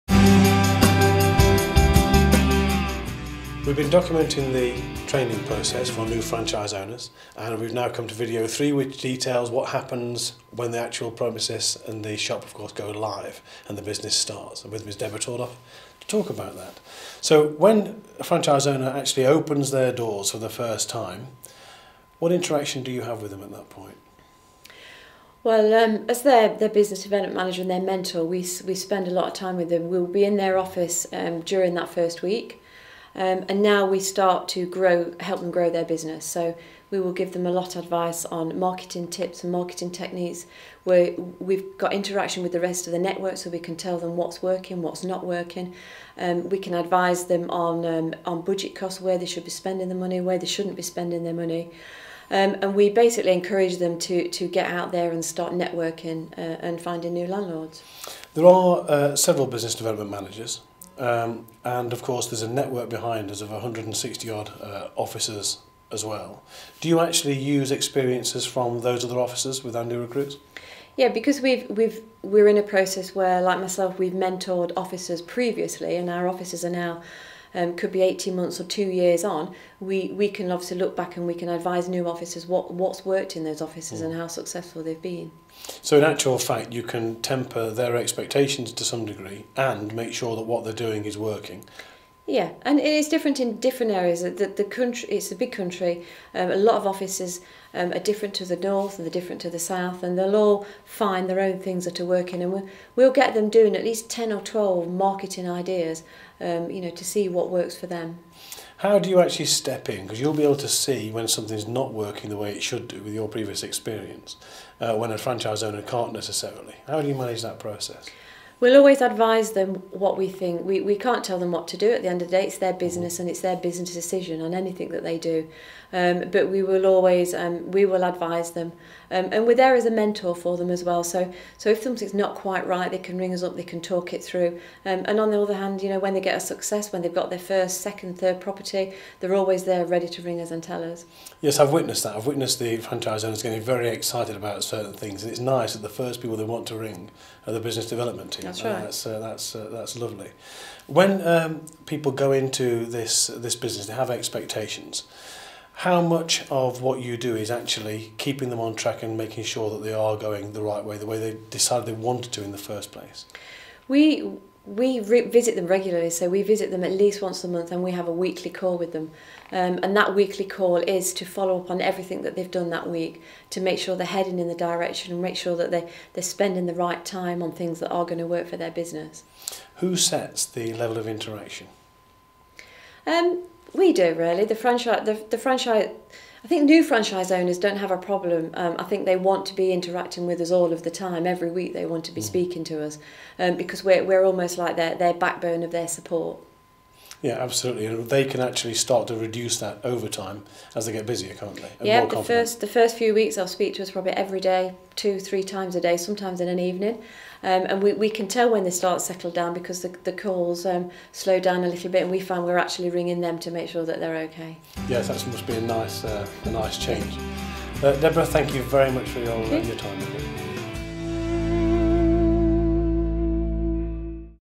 Franchisee training process - Part 3
In the final of 3 videos documenting the franchisee training process, Business Development Manager, Deborah Tordoff, talks about the long-term support and training from Belvoir.